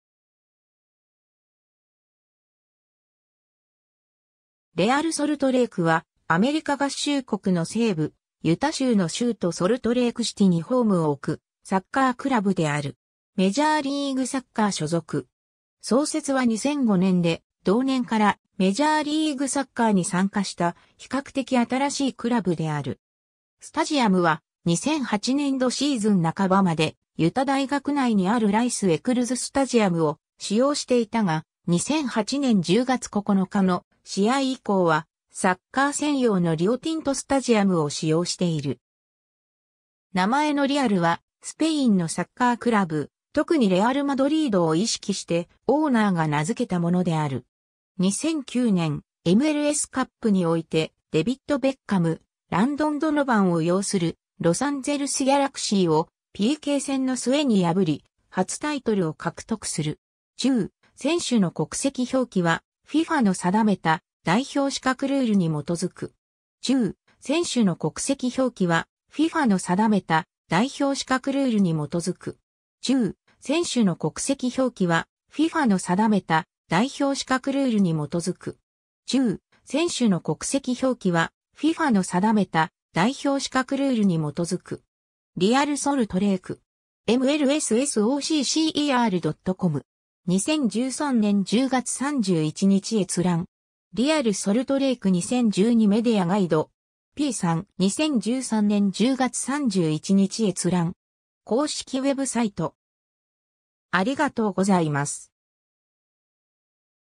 レアル・ソルトレイク
レアル・ソルトレイク（英語: Real Salt Lake）は、アメリカ合衆国の西部、ユタ州の州都ソルトレイクシティにホームを置くサッカークラブである。
メジャーリーグサッカー所属。
 創設は2005年で、同年からメジャーリーグサッカーに参加した、比較的新しいクラブである。
 スタジアムは、2008年度シーズン半ばまでユタ大学内にあるライス・エクルズ・スタジアムを使用していたが、2008年10月9日の試合以降は、サッカー専用のリオ・ティント・スタジアムを使用している。
 名前のRealは、スペインのサッカークラブ、特にレアル・マドリードを意識してオーナーが名づけたものである。
 2009年、MLSカップにおいて、デビッド・ベッカム、ランドン・ドノバンを擁するロサンゼルス・ギャラクシーをPK戦の末に破り、初タイトルを獲得する。
 ^ “Real Salt Lake”. mlssoccer.com. 2013年10月31日閲覧。
 ^ “Real Salt Lake 2012 Media Guide”. p. 3. 2013年10月31日閲覧。